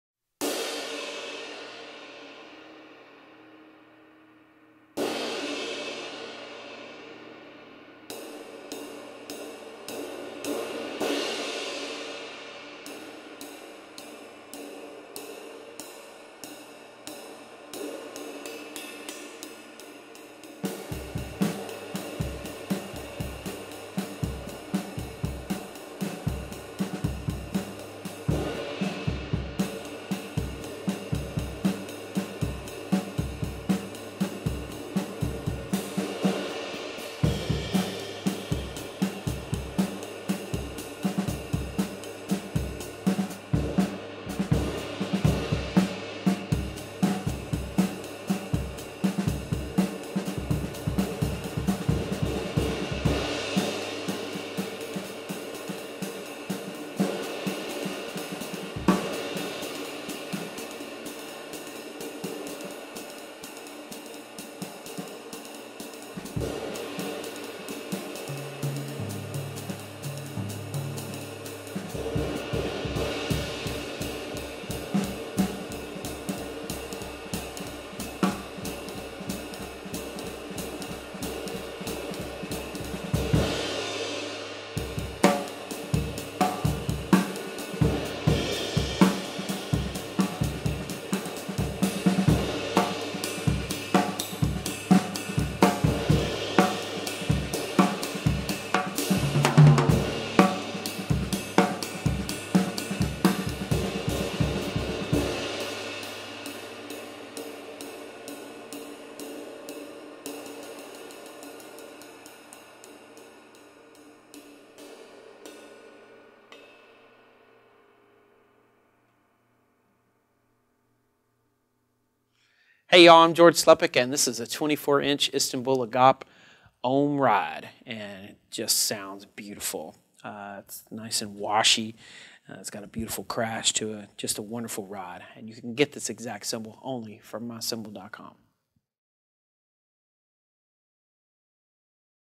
Istanbul Agop 24" Signature OM Ride Cymbal - 2786g (CBOR24-1050817Q)
*SOLD* Istanbul Agop 24" Signature OM Ride Cymbal.

Other gear used in this video:
• Cymbals (From drummer's L to R): Istanbul Agop 15" Traditional Light Hi Hat Cymbals, Istanbul Agop 20" Signature Mantra Crash Cymbal